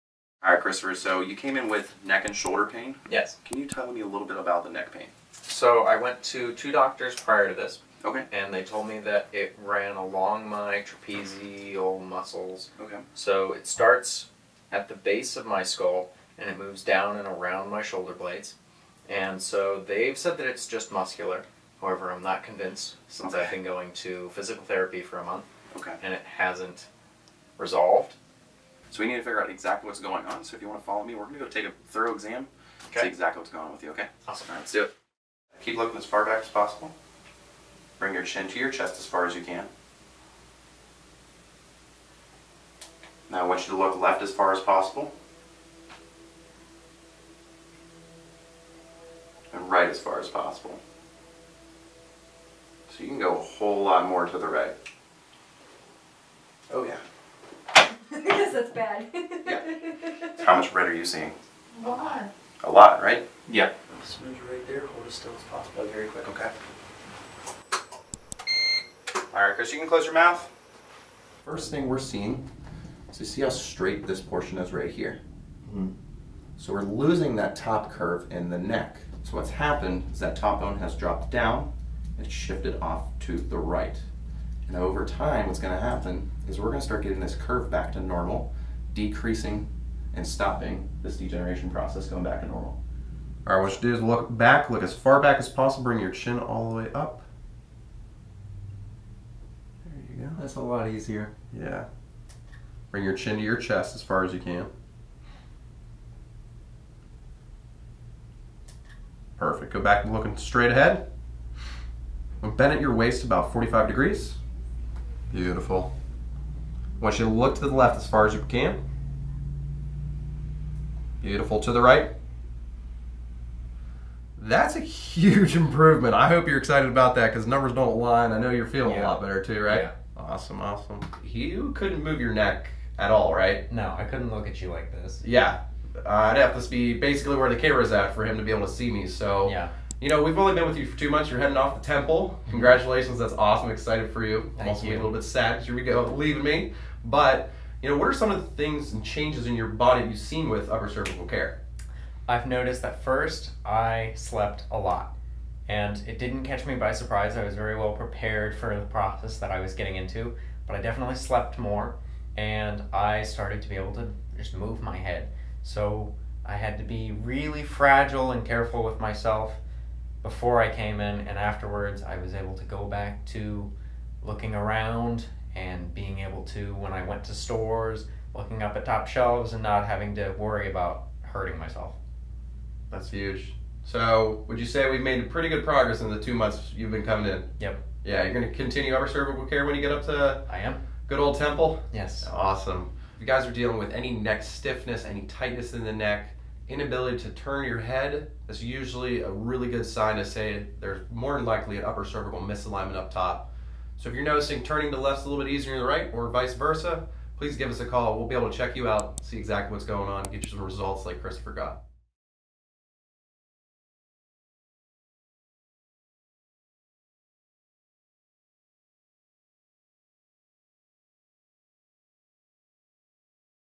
Christopher's Upper Cervical Journey
Take a closer look at Christopher's Upper Cervical journey. Since starting Upper Cervical Care, his neck stiffness has improved, and he is feeling like a healthier, young man!